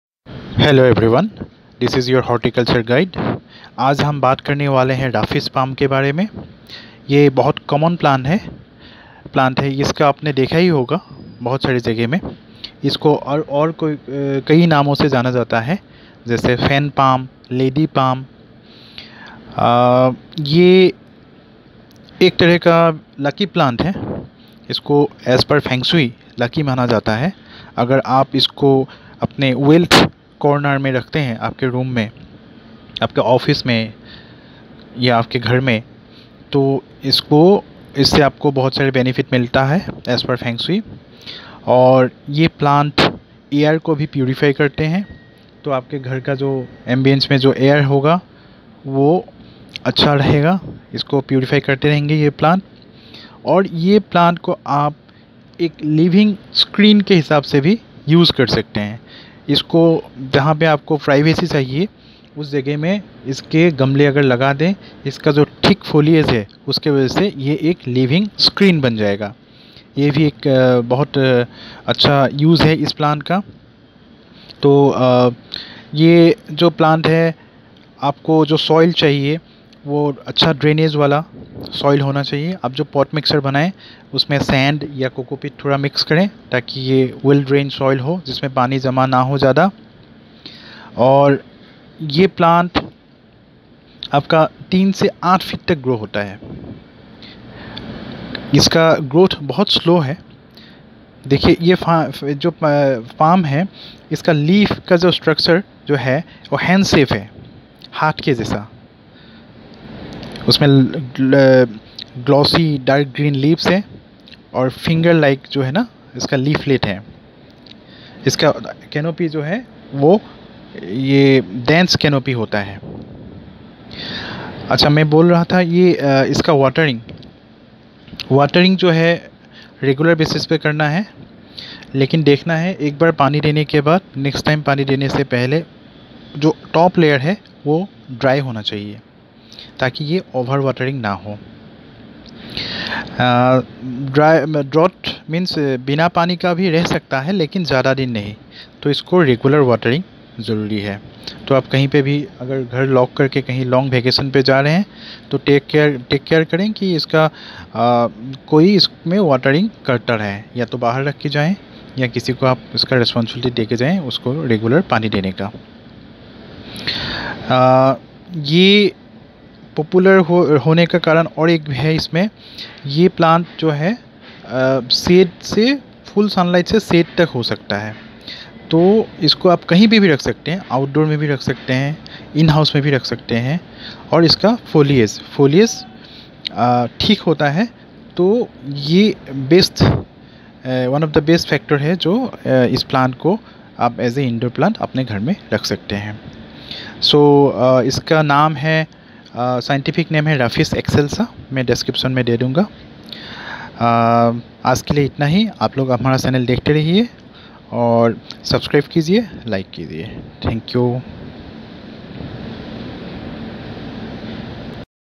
Raphis Palm, Lady Palm, Fan Palm
Looks, uses, maintenance & light requirements